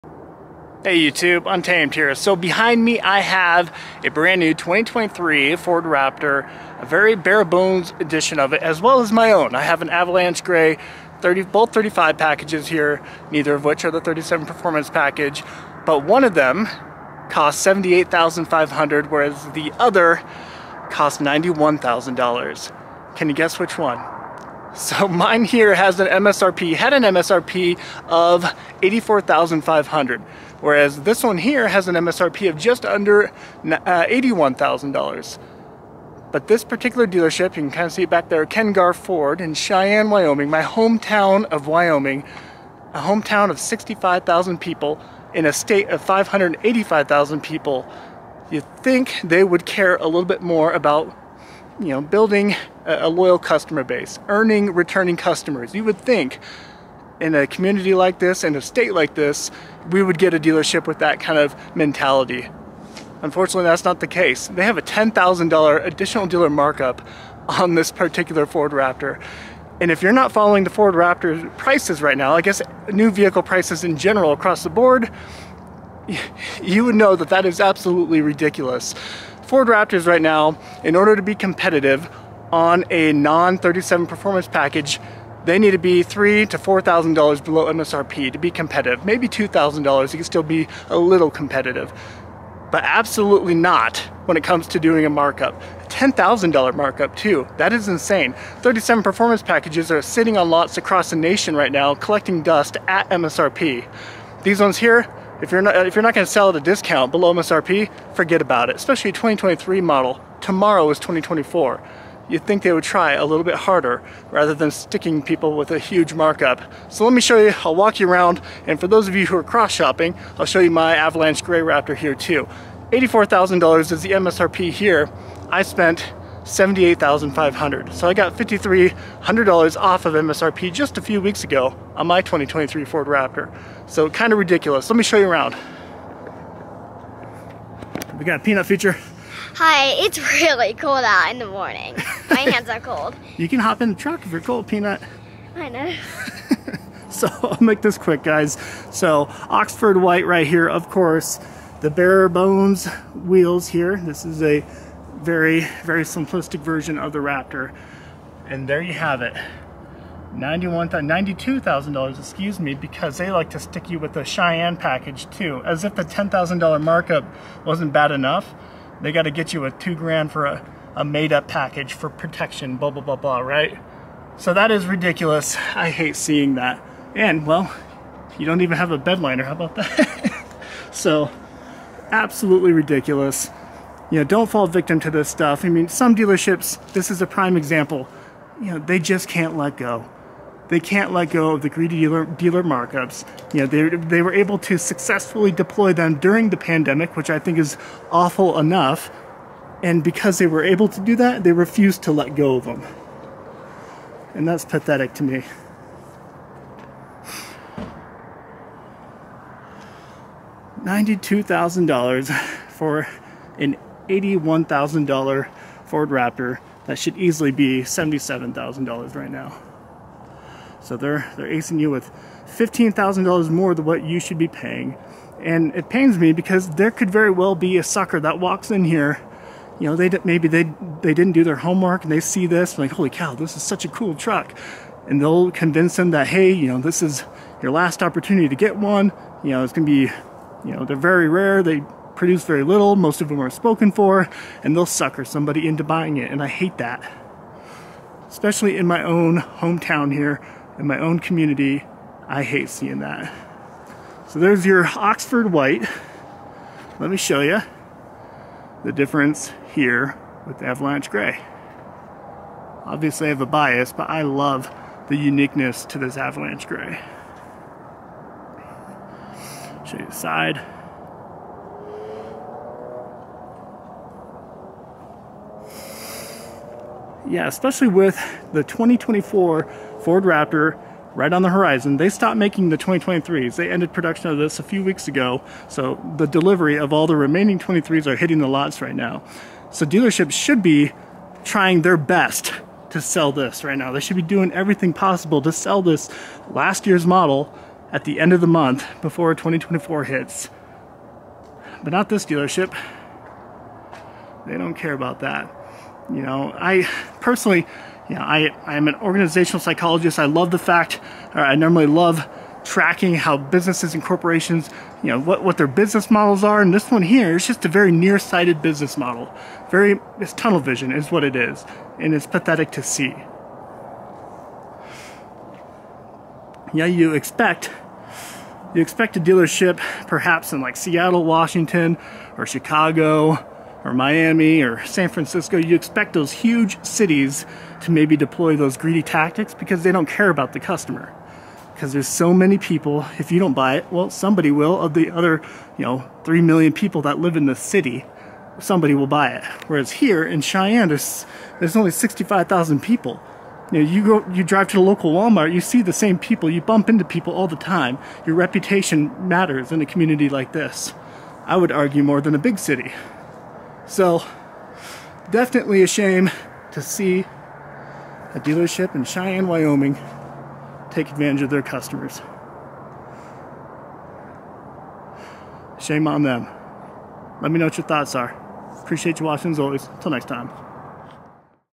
Some Dealers Just CAN'T LET GO of MARKUPS
It's insane how some dealers just REFUSE to let go of their greedy markups. I couldn't imagine being so blinded by greed to the point where I'm oblivious of what the market is. The new and used vehicle market is in shambles at the moment... yet here we are, looking at a 2023 (a year old tomorrow) Ford F150 Raptor base model with a $10K MARKUP!? Blows my mind. In little ole' Cheyenne Wyoming, this dealer is trying to take advantage of their customers. Showing zero regard for the consumer... only for their own profits and gain. Smaller dealerships in small town USA tend to care about returning customers and customer loyalty. Reputation is everything in small towns. Why would they try to milk someone of $10K more than MSRP?

I expect this type of greedy dealership behavior from dealers in populated cities like Seattle, Chicago, Miami, etc... but not Cheyenne, Wyoming.

Personally, I will NEVER buy from a dealership that deployed ADMs/Markups. I will also spread the word to my friends and family to AVOID dealers like this. And I know I'm not alone in that mentality. People remember the bad taste they had in their mouths dealing with greedy dealerships like this, they won't forget. That'll be a huge ripple-effect that'll last years, especially in smaller communities like mine. Charging markups is such a near-sighted business model.

What re your thoughts? Are you still seeing dealerships deploy ADMs like this? If so, what are the amounts you're seeing and on what vehicles?

-Stay Untamed...